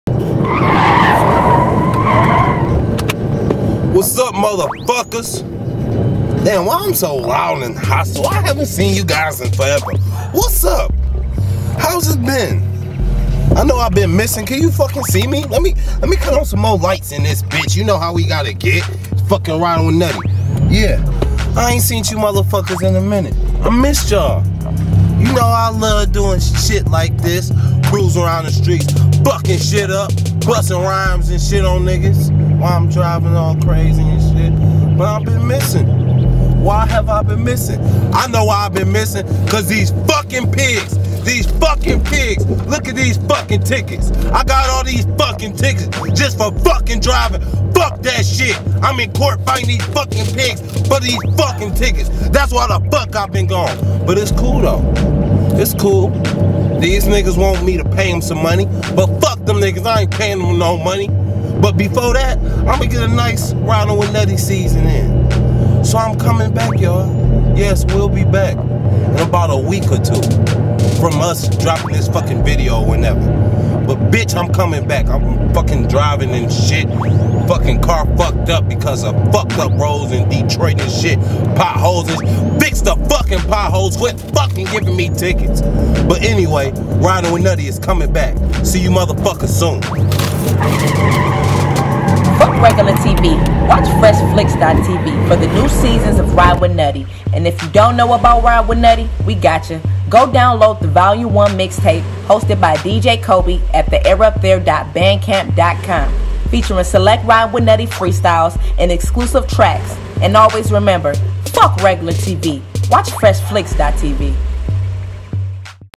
Ridin Wit Nutty is Back
Aww Man. One of our greatest series is bak! Ridin Wit Nutty. The show in which our own Nutty kidnaps rappers and makes them freestyle while he drives that Nutty way he does. I hope he dosen't get pulled over this season.